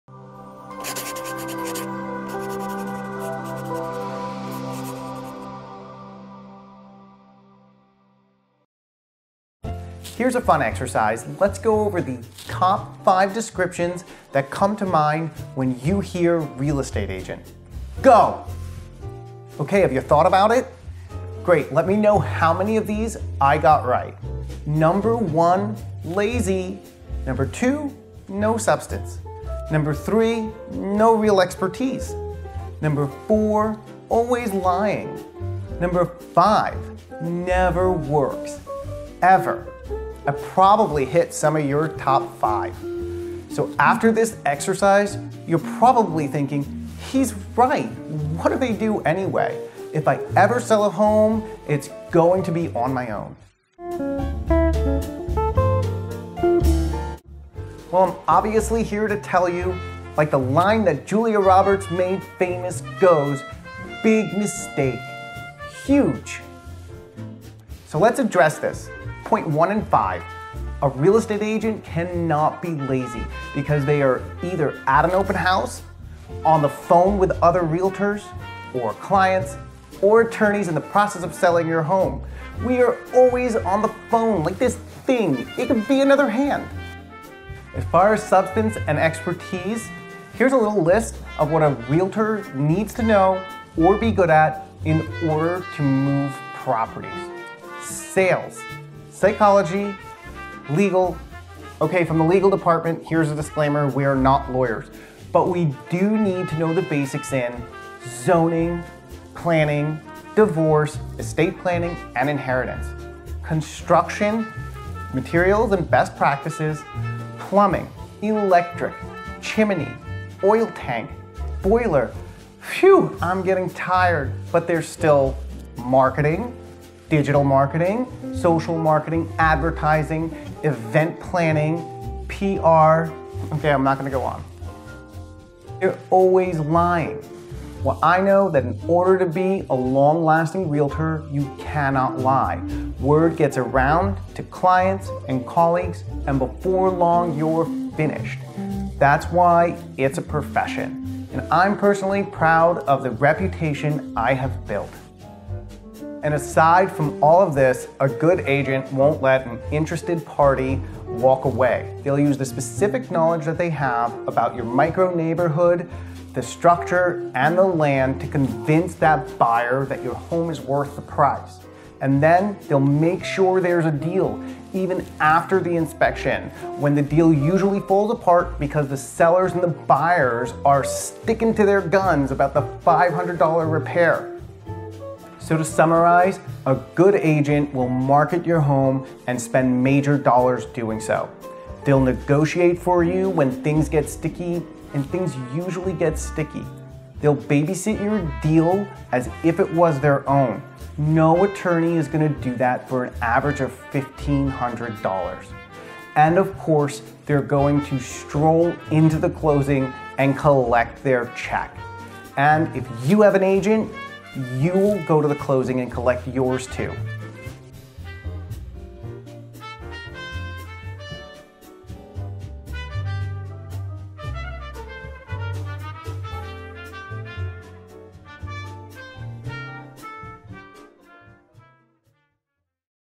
Top 5 Descriptions that Come to Mind When You Hear Real Estate Agent
Happy holidays from the Murad Team, we hope this message lightens your mood and gives you something to think about. Everyone needs a little holiday cheer ;)

The Murad Team are agents with Remax Elite Associates.